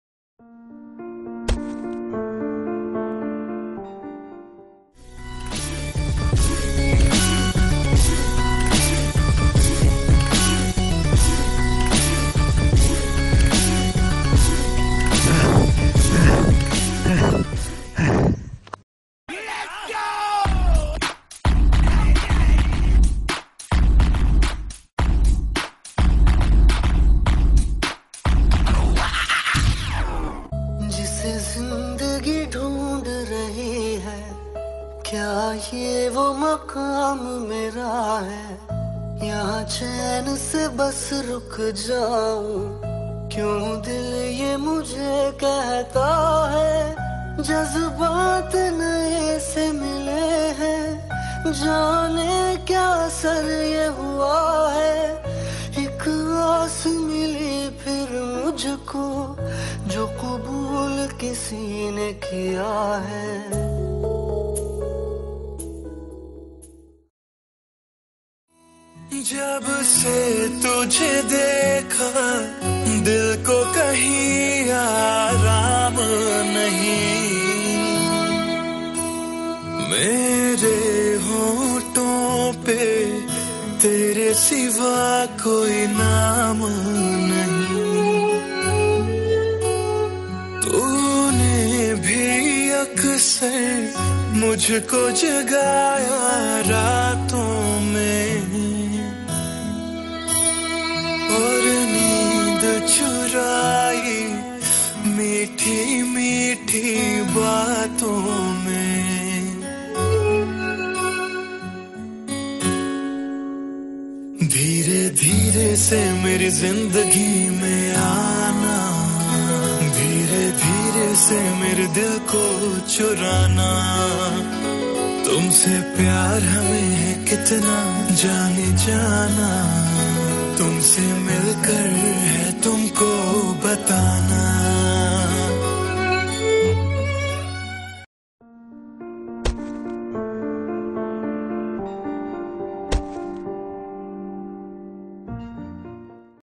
Krumping and lyrical hip hop
Our first duet video
Choreograph by : Sajid Ali
In association of Balajee academy of talents.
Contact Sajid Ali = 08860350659